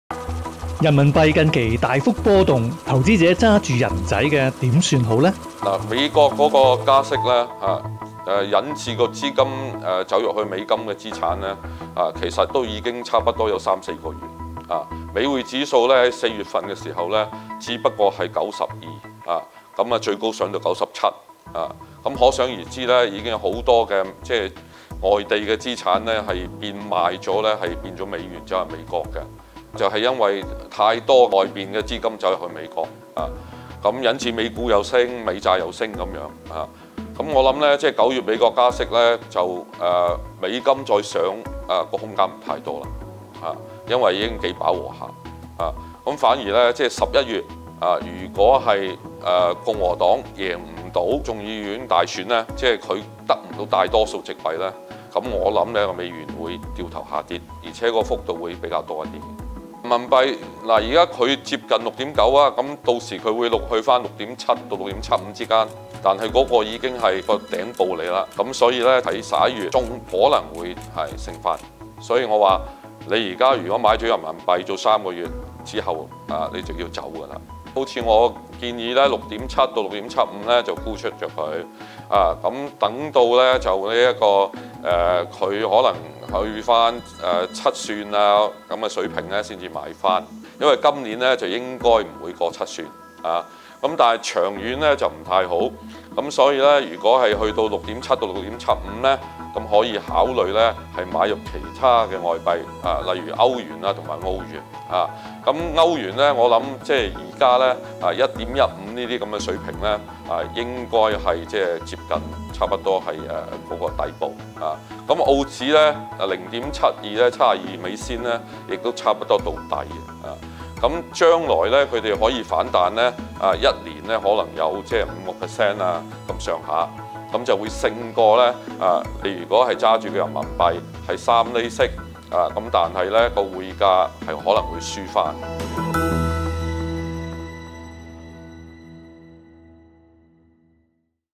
人幣見6.7即沽 揸歐元澳元
人民幣兌一美元近日接連跌穿多個關口，中國人民銀行上周五終再度出手托住匯價才告企穩。星展銀行(香港)財資市場部董事總經理王良享接受本報訪問時預期，中美貿易戰短期難以解決，人民幣就算反彈，料在6.7水平見頂，建議可先行沽貨，轉揸歐元及澳元。

universalproductionmusic